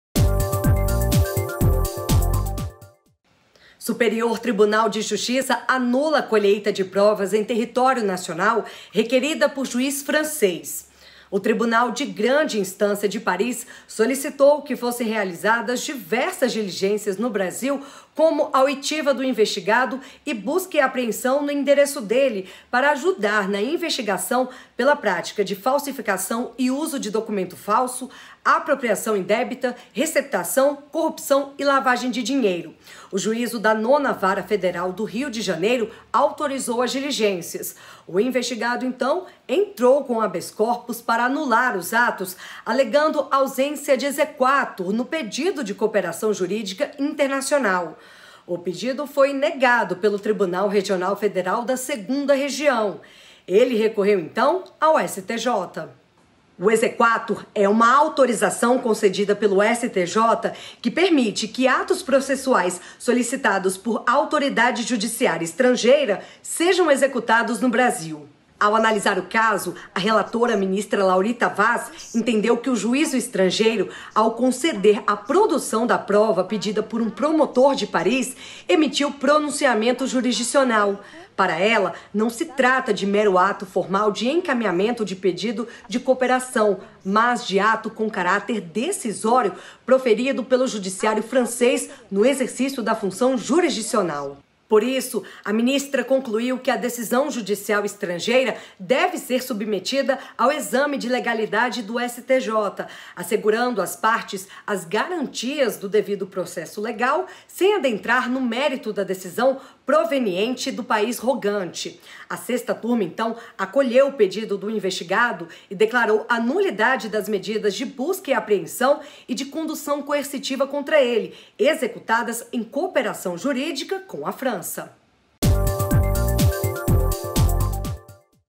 Sexta Turma anula colheita de provas em território nacional requerida por juiz francês 09.07.20
​Por falta de exequatur do Superior Tribunal de Justiça (STJ), a Sexta Turma acolheu o pedido de um investigado e declarou a nulidade de medidas de busca e apreensão e de condução coercitiva contra ele, executadas em cooperação jurídica com a França. O exequatur é uma autorização concedida pelo STJ para o cumprimento de cartas rogatórias no Brasil, como prevê o artigo 105, I, "i", da Constituição Federal.